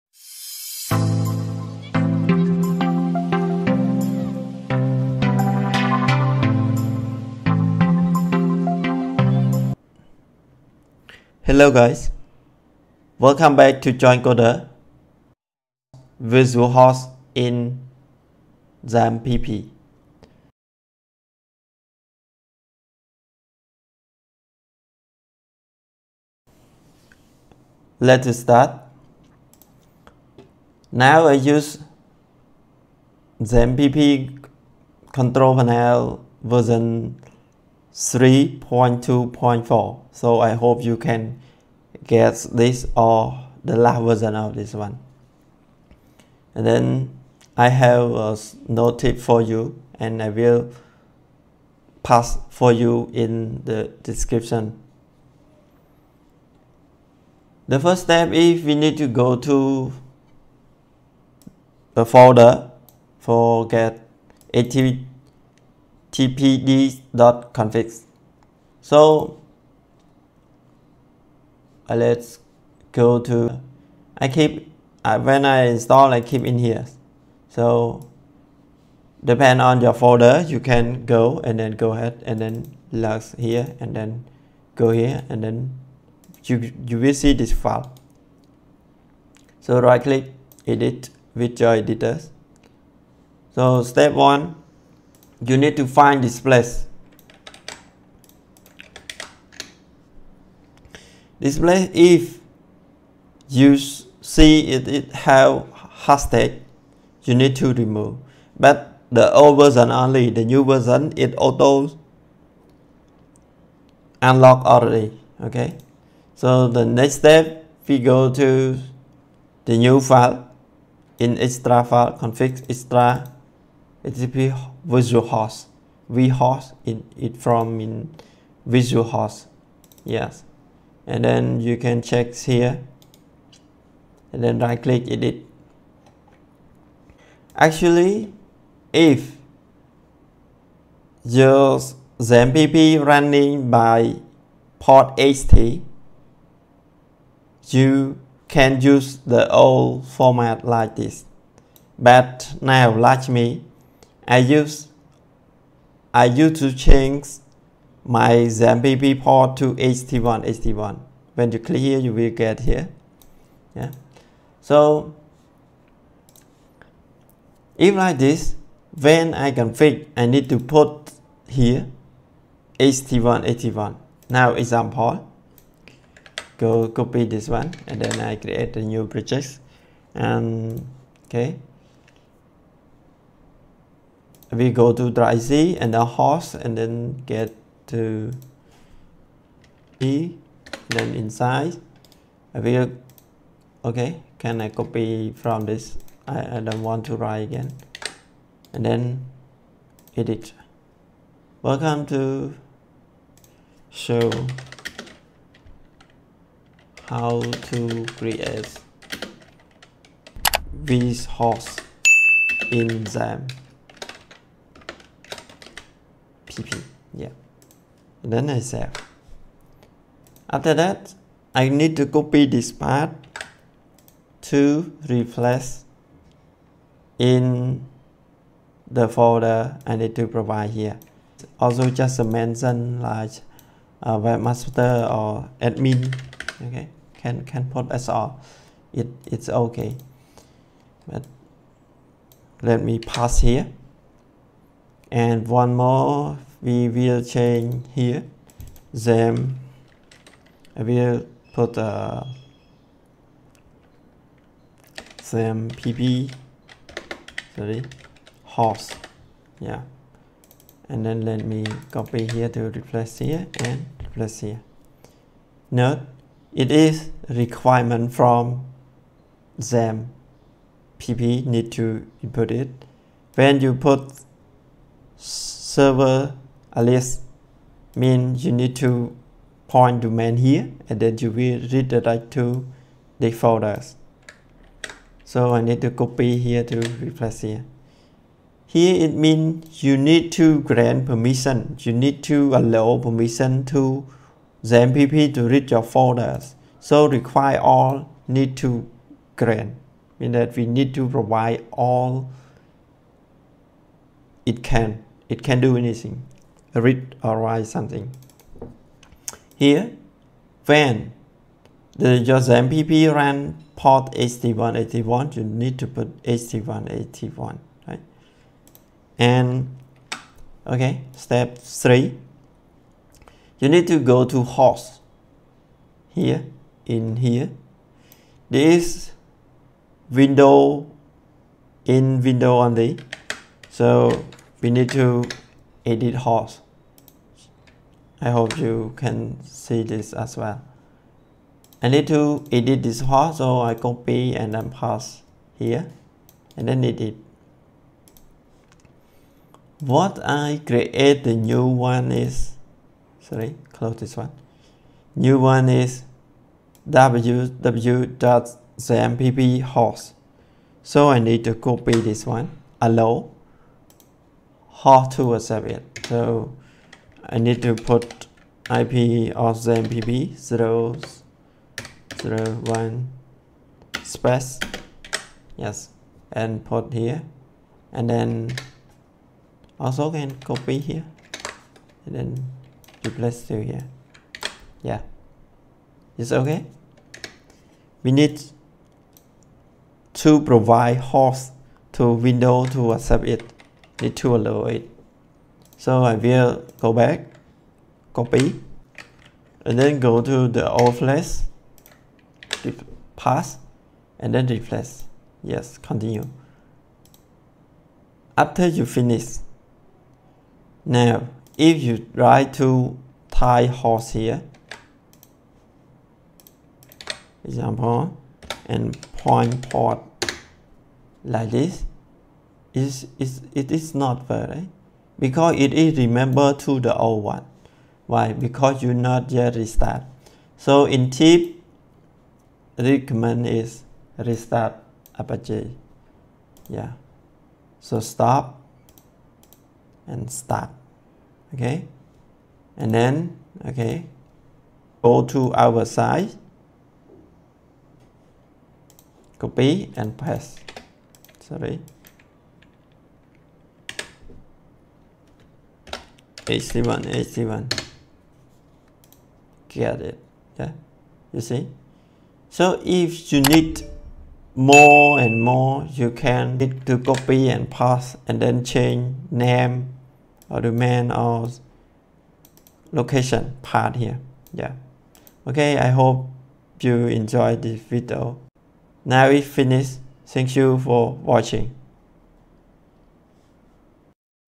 How to create vHost in Xampp | Xampp configure
Step 1:
Open httpd.conf file present in C:\xampp\apache\conf\httpd.conf
Remove the #(hash) sign present to include the “httpd-vhosts.conf” file in httpd.conf file.

Step 2:
  Create a virtualhost file. Open “httpd-vhosts.conf” file. And copy the below lines of code.

    DocumentRoot "C:\Hosting\youtube"
Step3:
Open C:\Windows\System32\drivers\etc\hosts
Add the below line at the end of file.

Share your idea to the world . Thank for your comment